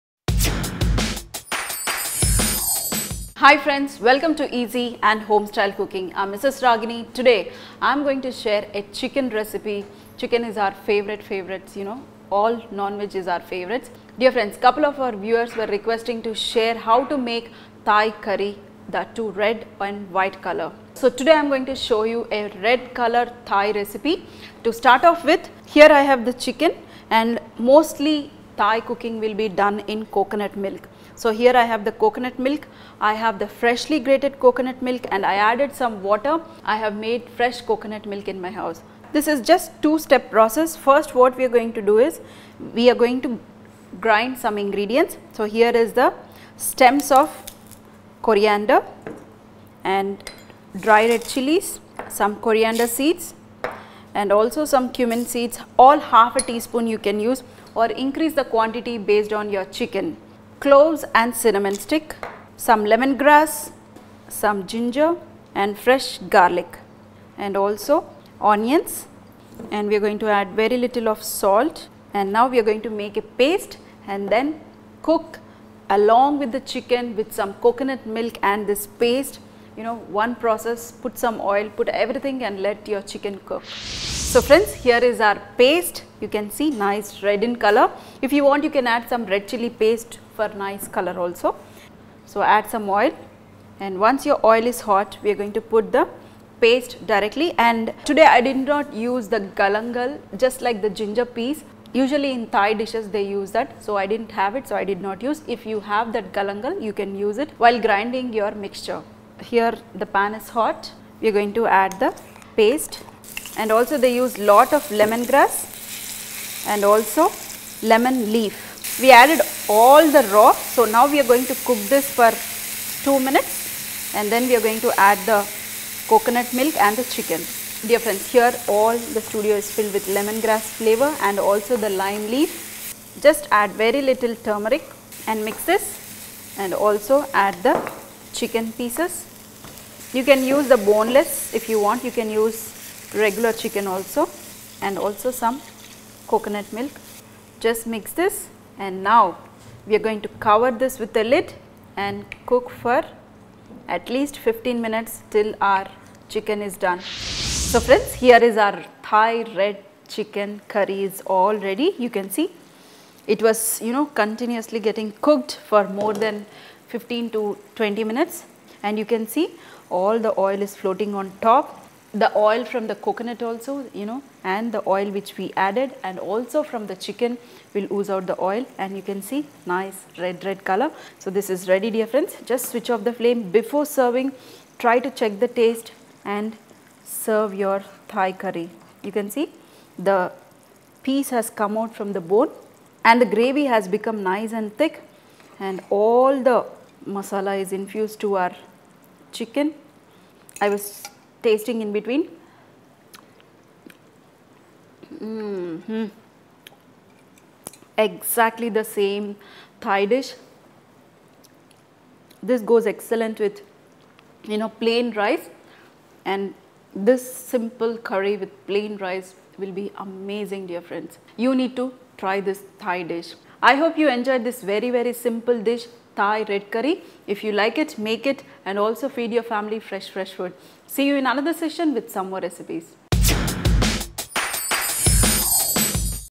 RED THAI STYLE CHICKEN - Mrs Vahchef
This is a quick and easy Northern Thai chicken curry prepared  with chicken,  spicy paste and coconut milk.

INGREDIENTS:

Coriander stems  5-10 gms
Dry red chillies 3-4 nos

METHOD:

1. Take a mixie jar and add coriander stems, dry red chillies, coriander seeds, cumin seeds, cloves, cinnamon stick, lemon grass, ginger, garlic, onion, salt and blend into red colour paste.

2. Heat oil in a pan and add red colour paste, lemon leaves, turmeric powder, chicken and saute it.

3. To it add coconut milk and place lid and cook for 15- 20 min with lid on it.

4. Now the red thai style chicken is ready to serve.